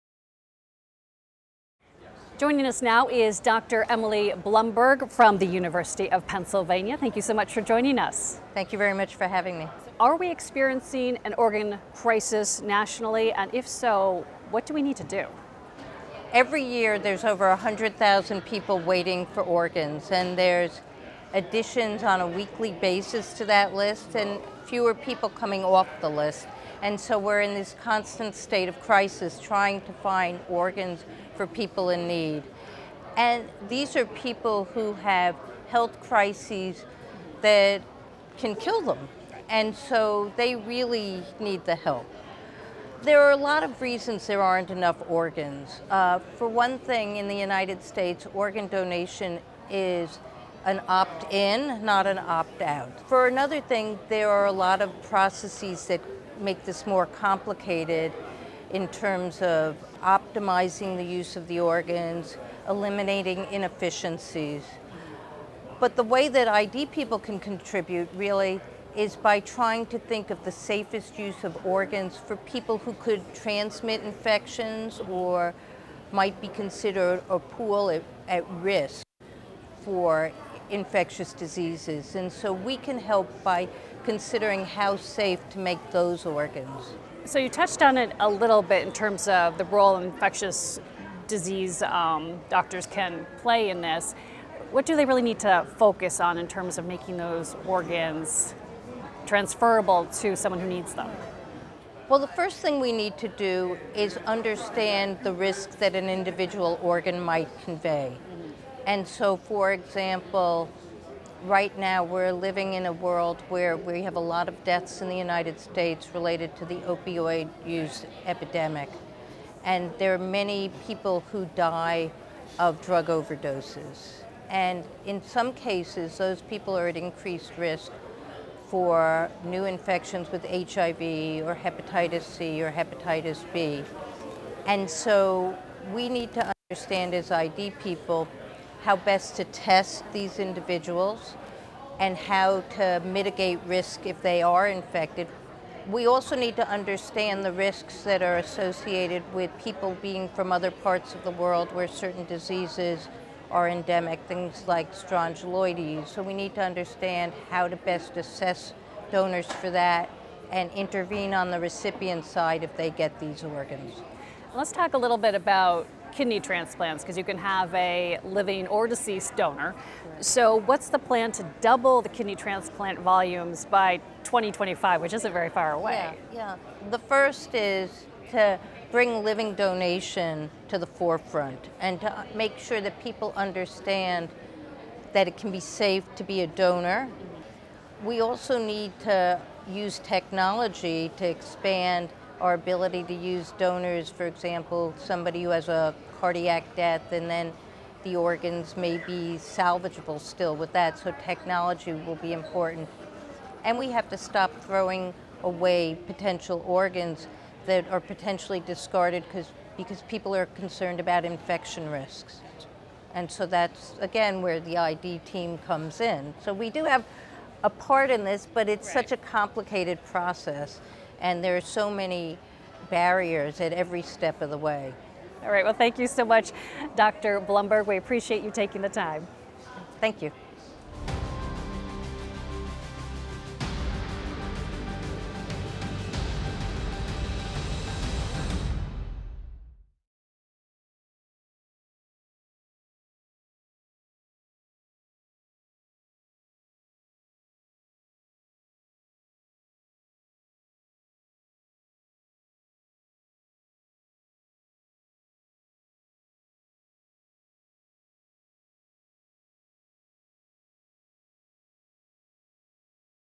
The Organ Shortage Crisis, IDWeek 2023
At IDWeek 2023, Dr. Emily Blumberg from the University of Pennsylvania provides insight on solving the organ shortage crisis.